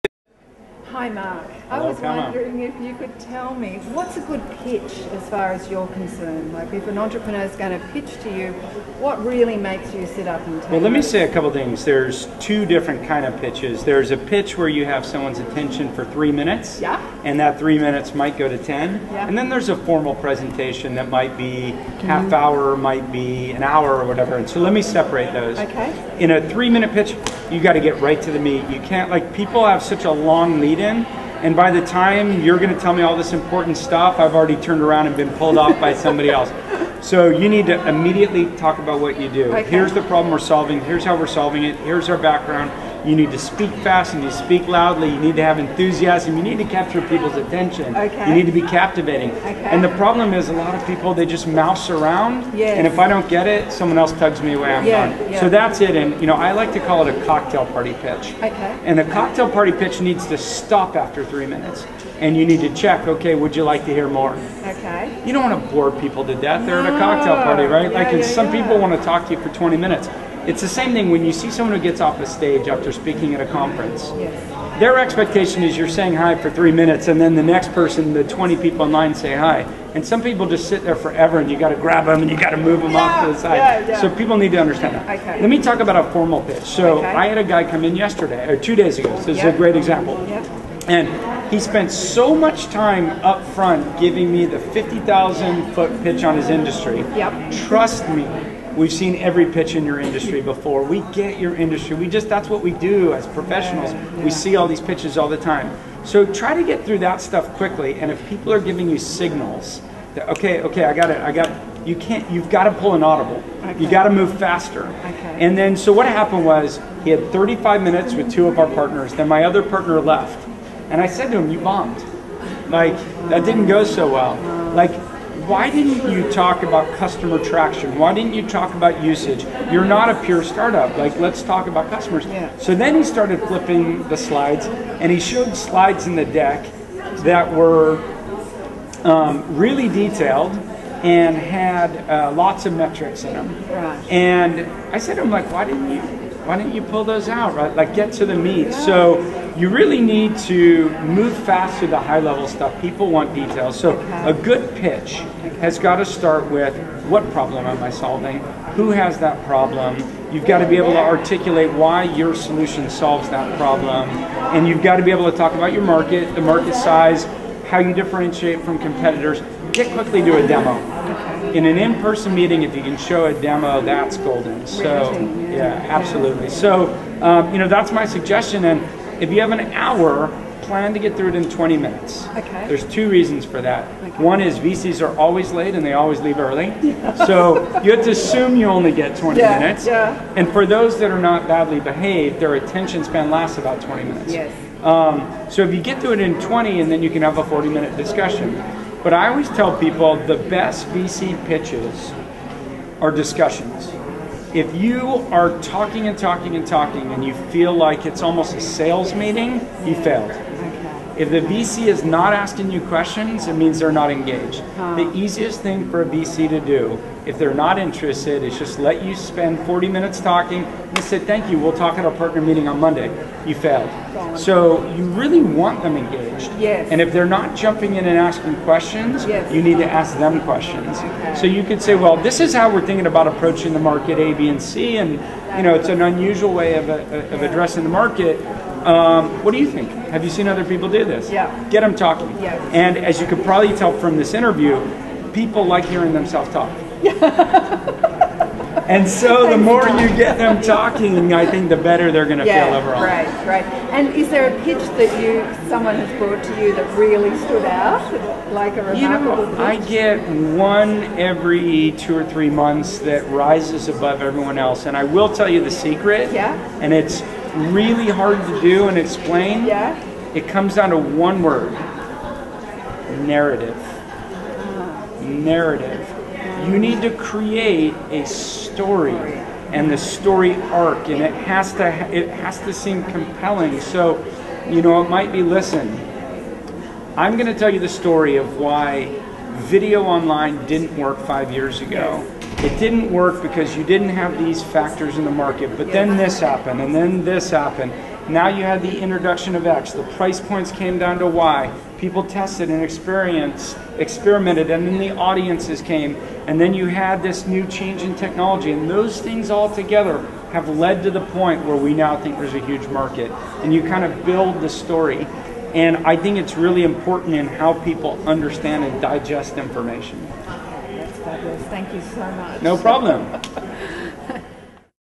Mark Suster, GRP: the Cocktail Party Pitch @WITI San Diego 2012
Mark Suster, Partner GRP tells me about how to do the Cocktail Pitch & more.